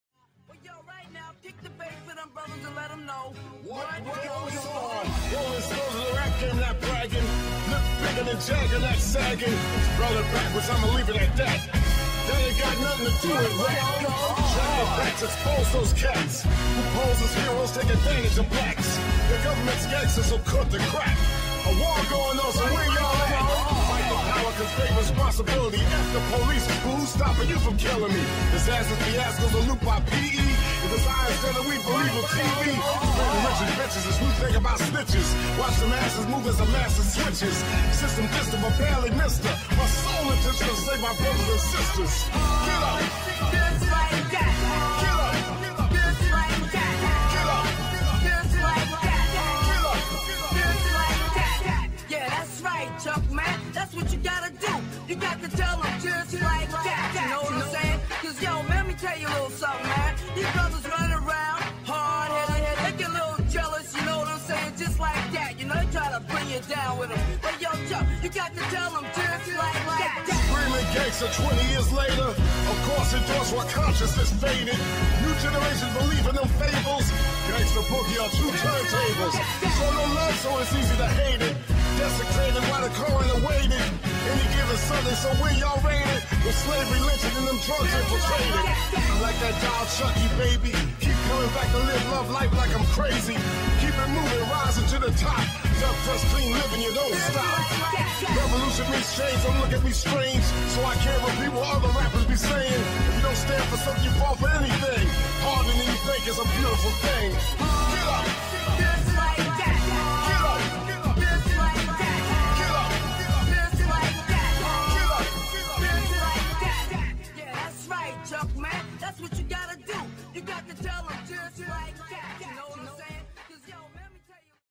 Defensive Boxing - Harder Than You Think
nothing crazy, just good music coupled with the sweet science of the sport i love.. boxing